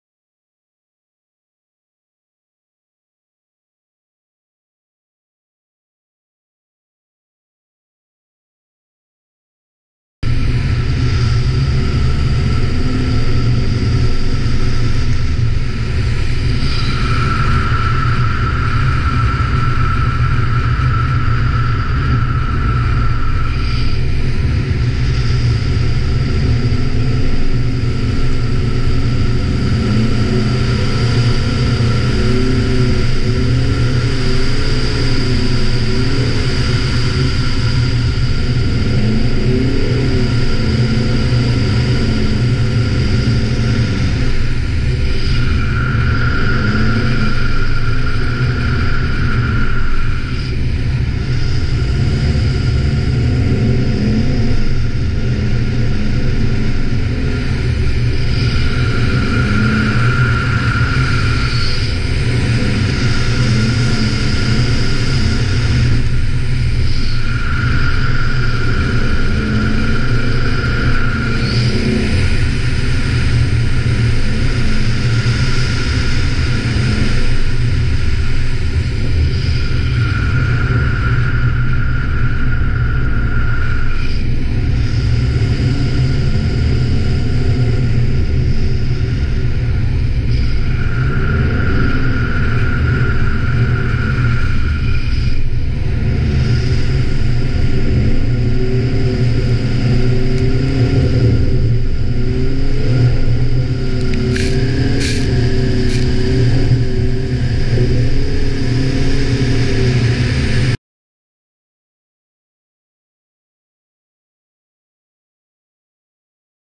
Volvo V90 Cross Country 2017 - Essai sur lac gelé
Embarquez à bord de la nouvelle Volvo V90 cross Country, pour un tour de circuit de glace avec des pneus cloutés homologués route. Puis retrouvez l'essai complet et les photos